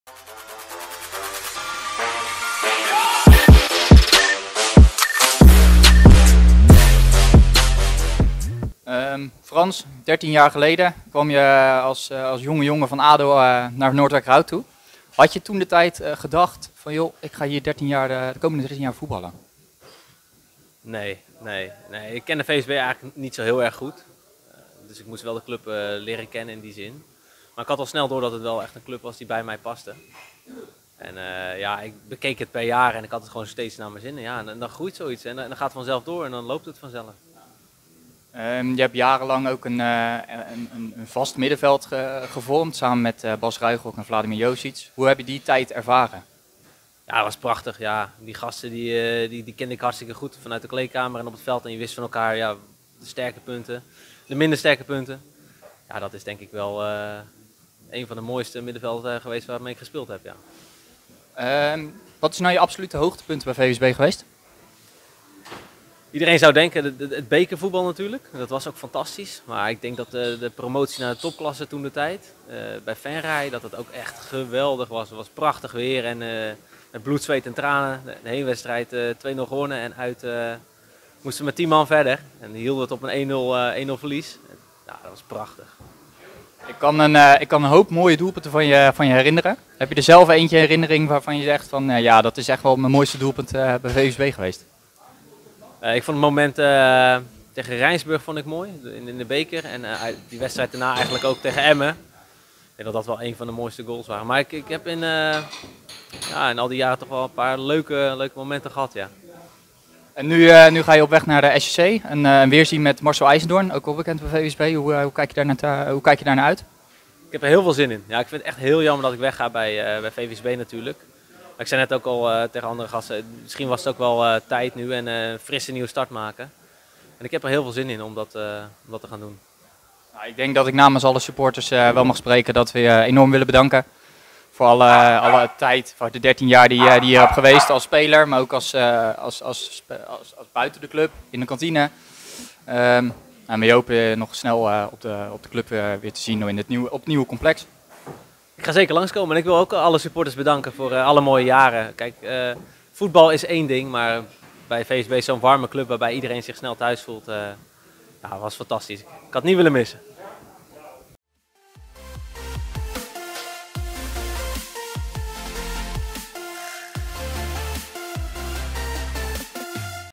In gesprek met Frans van Niel
Ook Frans gaat VVSB verlaten na 13 seizoenen , op het laatste uitje van VVSB afgelopen zaterdag hebben we dan ook geen definitief afscheid genomen van hem.  Frans komt volgend seizoen nog een keer afscheid nemen van het thuispubliek bij een wedstrijd van het eerste  zoals het hoort bij een clubicoon.